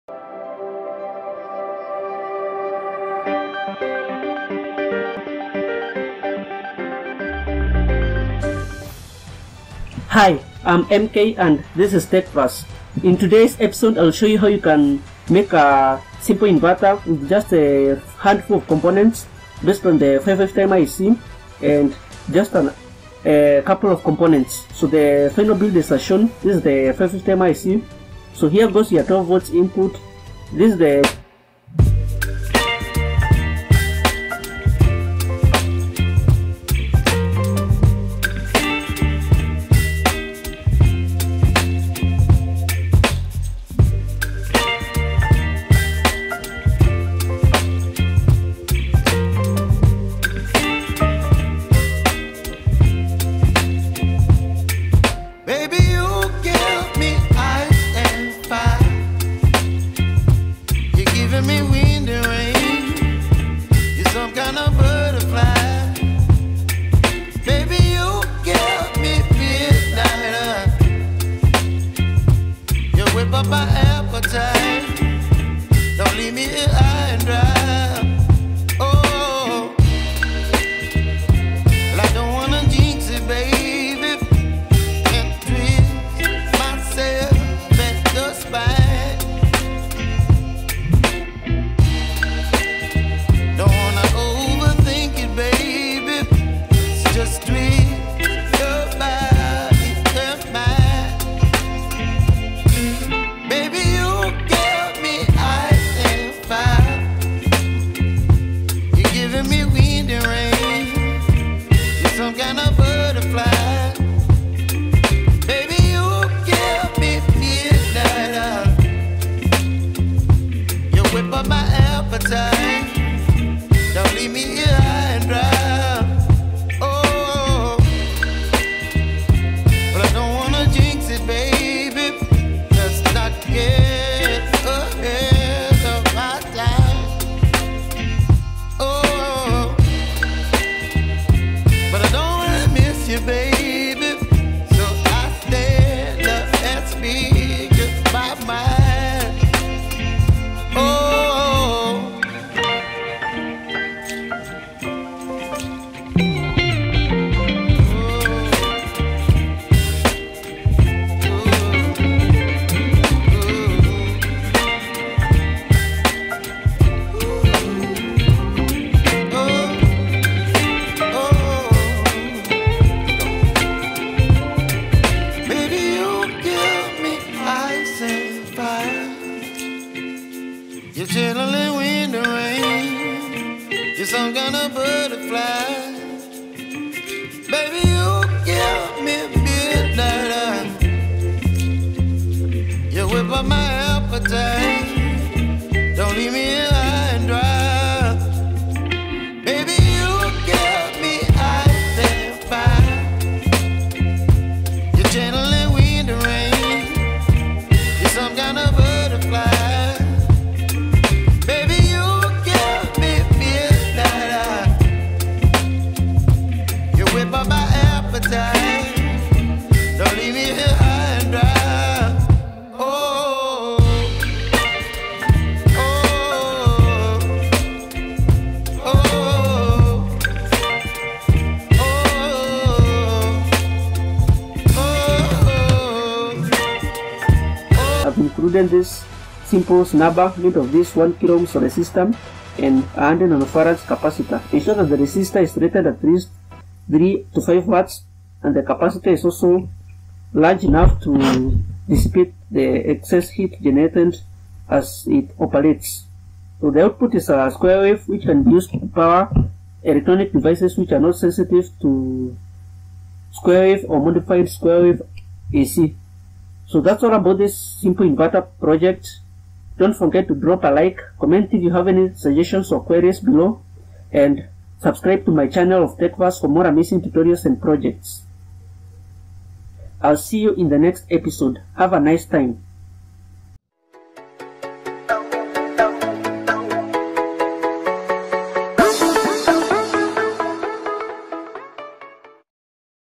How to make a 12V to 220V Inverter with NE555
Hey guys, in this video I'll show you how to make a rather simple inverter with a 555 timer and a handful of other components. The feasible power is about 360 to700W with only two Mosfets. It can however, be upgraded for more power.